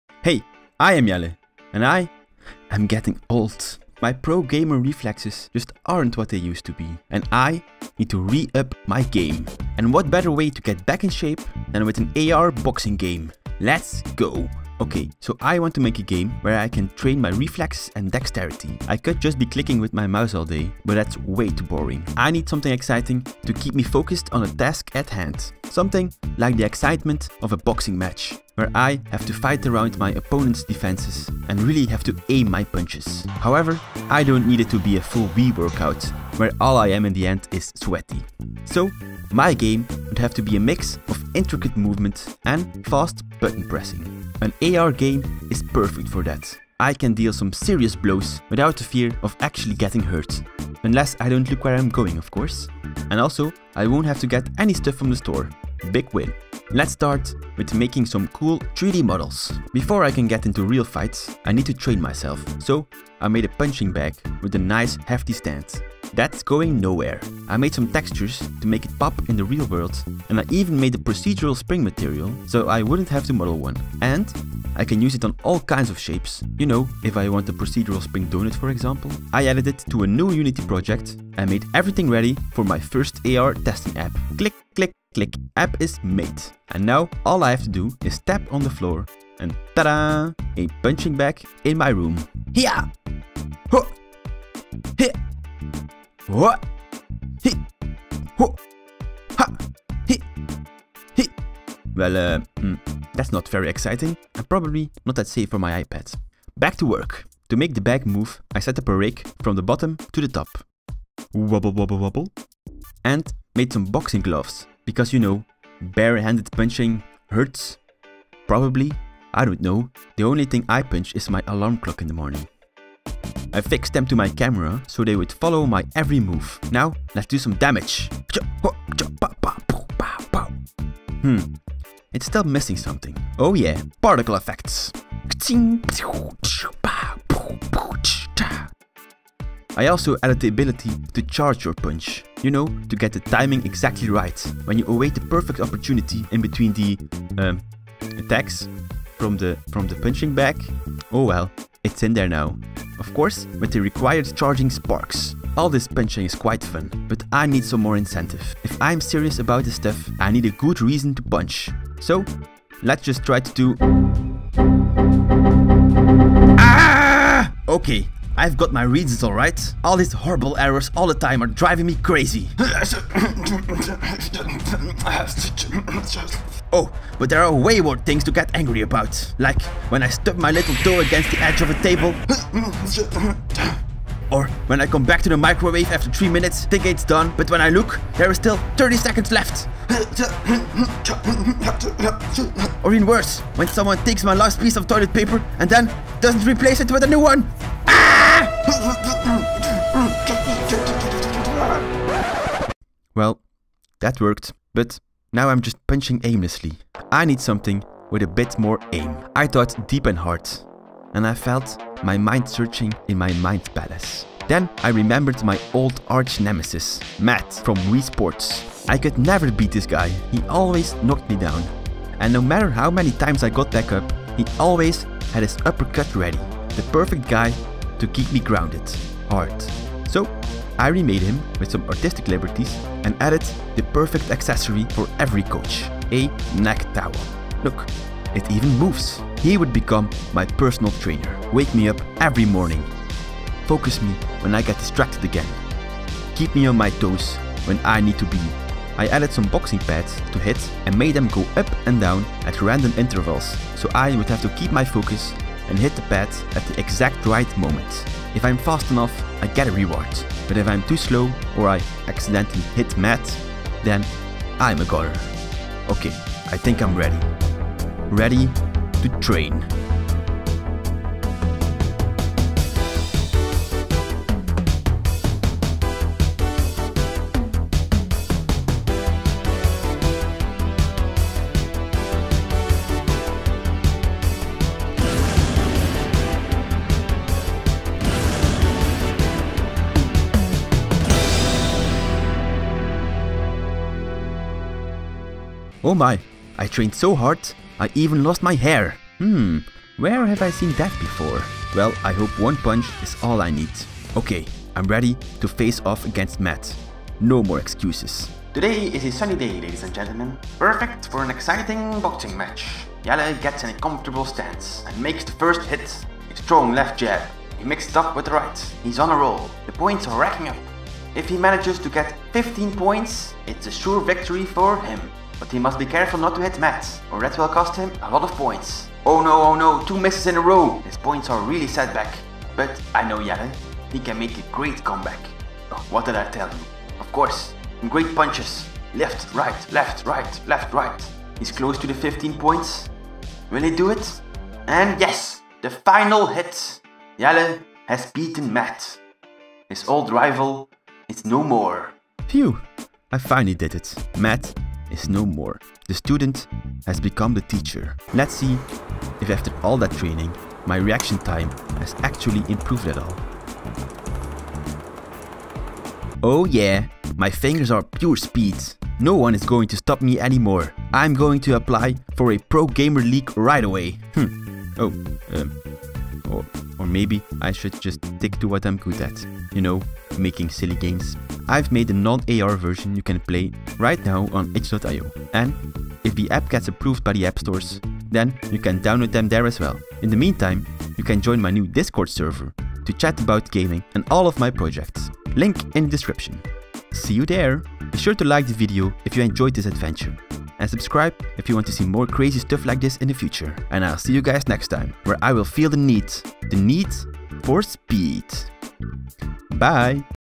Making an AR Boxing Game so I don't Get Fat
I made an AR boxing game in Unity so I won't get fat!

I used AR foundation in Unity to create a game where I can place Being objects or people in the real world to fight against!

Follow me everywhere!
And loads more! Just google me ;)

Programs used:
Blender, Unity, Visual studio, Procreate